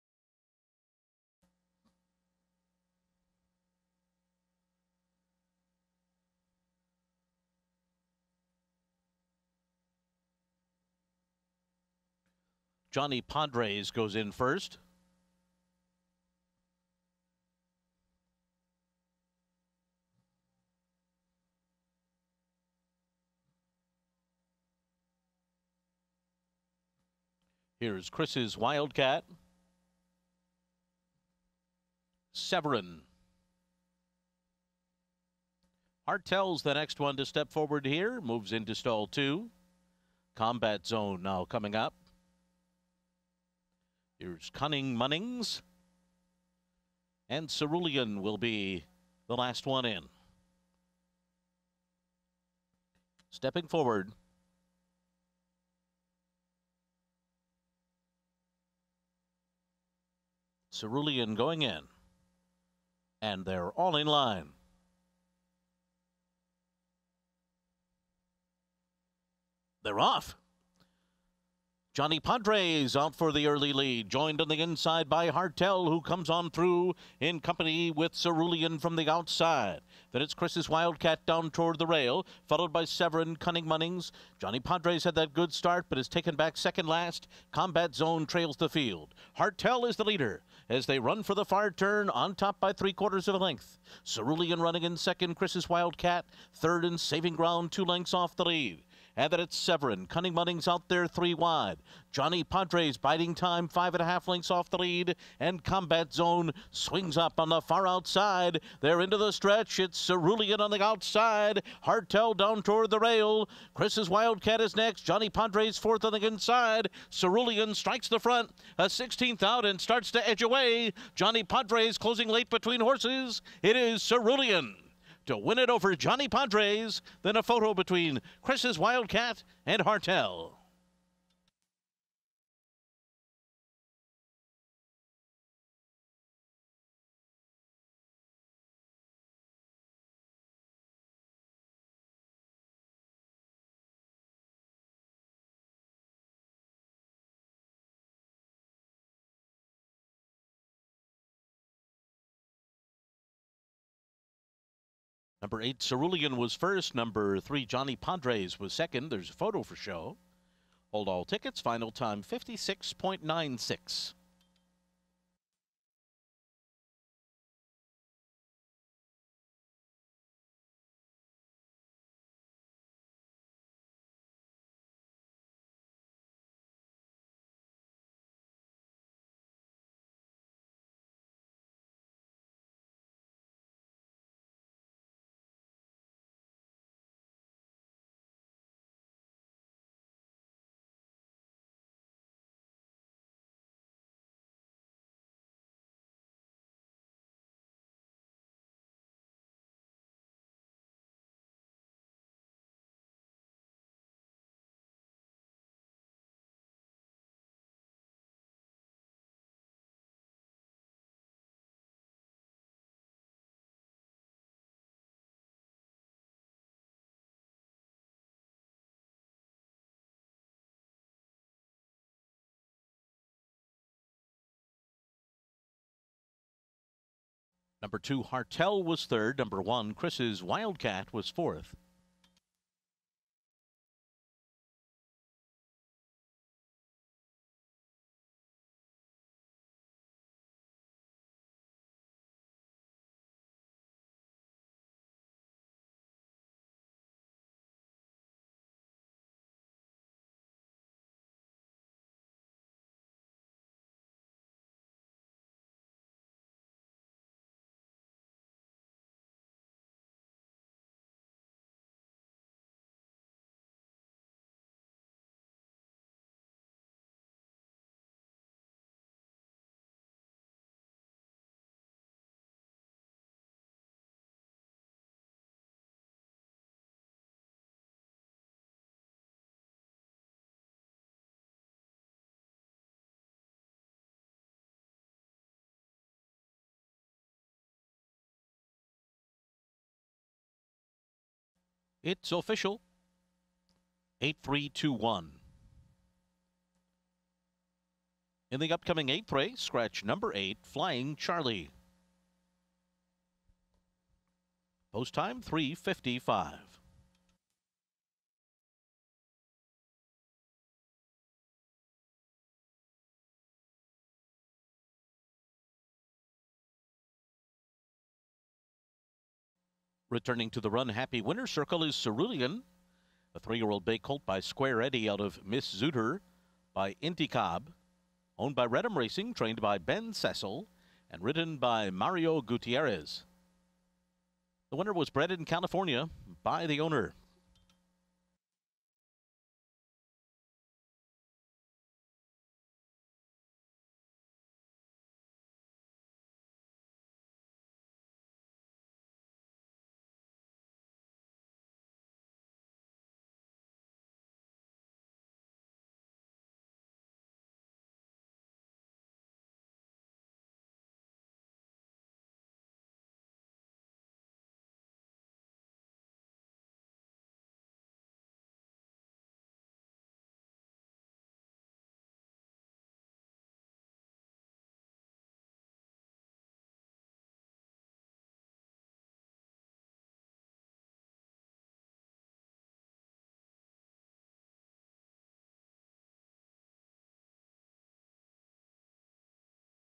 Caerulean wins race 7 at Del Mar 11/21/20
Top Finishers:
#8 Caerulean
#3 Johnny Podres
#2 Hartel
#1 Kris' Wild Kat